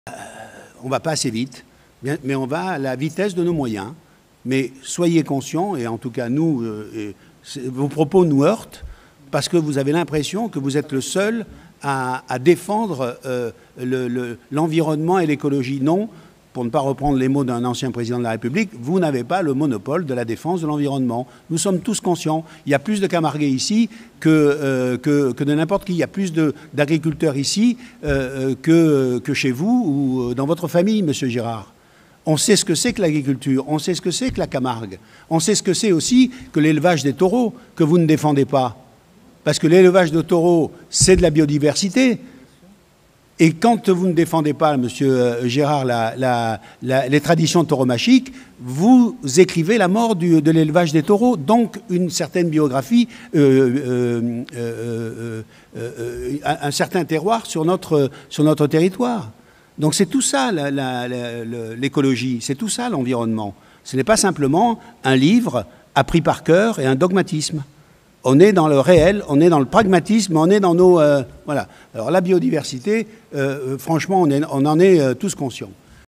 22 février 2023 – Conseil communautaire - 2
Réponse de Patrick de Carolis à Cyril Girard concernant le Rapport développement durable 2022 de l'ACCM